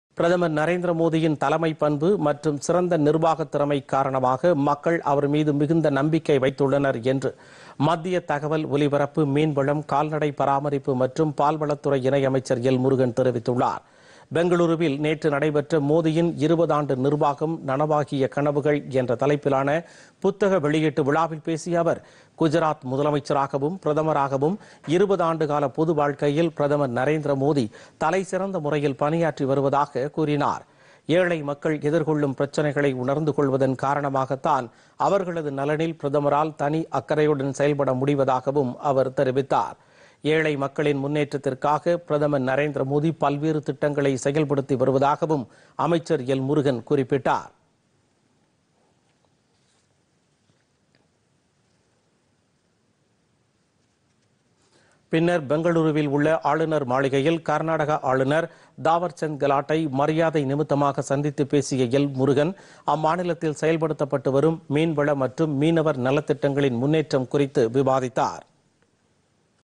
புத்தக வெளியீட்டு விழா
பிரதமர் நரேந்திர மோதியின் தலைமை பண்பு மற்றும் சிறந்த நிர்வாகத்தால் மக்கள் அவர்  மீது மிகுந்த நம்பிக்கை  வைத்துள்ளனர்- புத்தக வெளியீட்டு விழாவில் மத்திய இணை அமைச்சர் எல் முருகன் பேச்சு